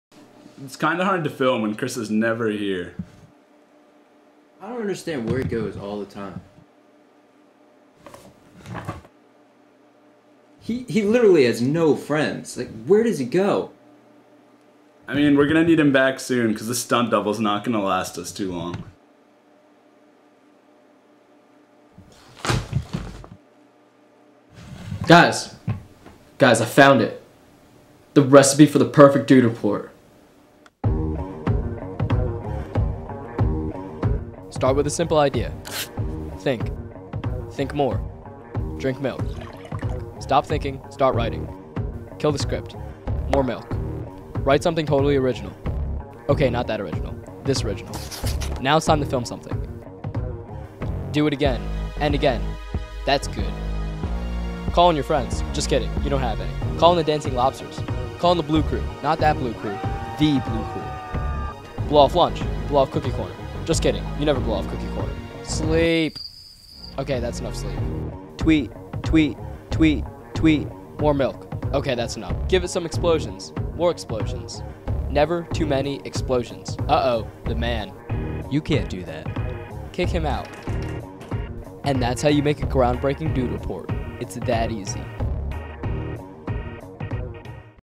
Dude Report - Special Recipe
The Dudes find a special recipe for the perfect dude report.